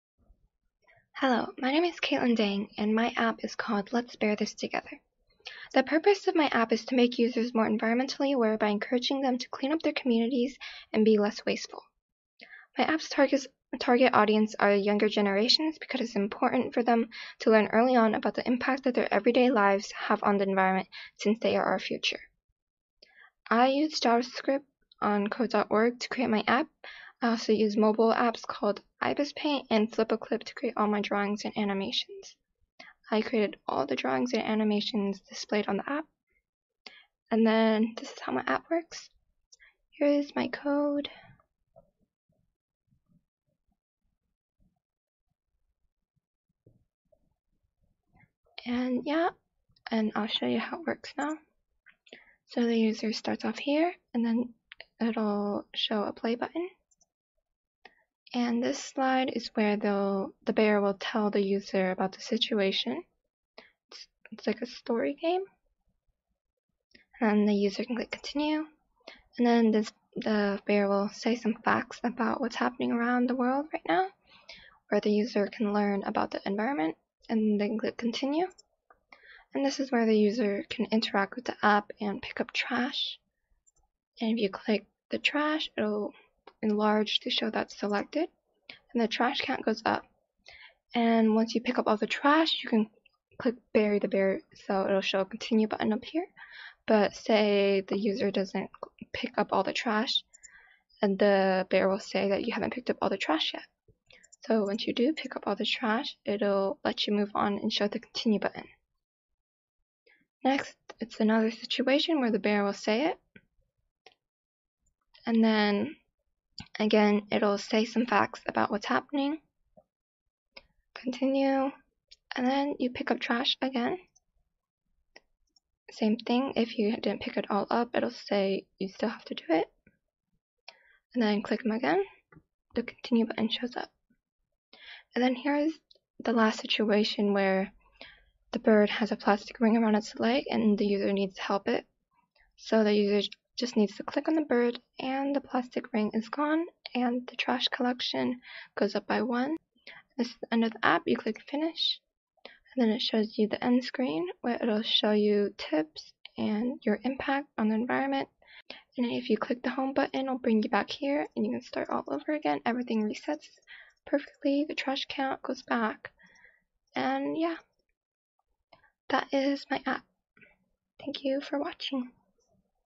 “Let’s Bear This Together!” - Congressional App Challenge 2022
Entry for the Congressional App Challenge of 2022. Representing District CA32 - Grace Napolitano.